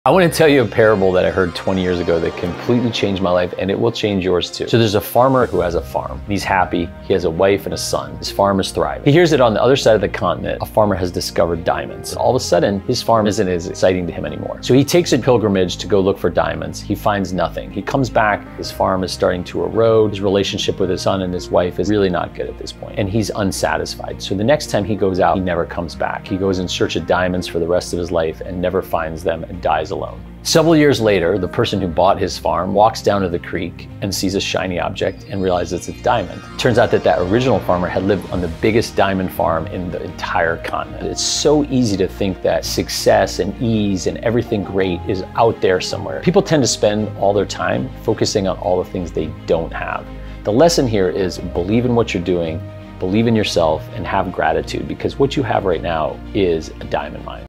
A Secret Most Won't Tell You
It's easy to get caught up in the trappings of wanting more. The key to success? Believe in what you’re doing, believe in yourself, and have gratitude.

Share this with someone who needs to hear it!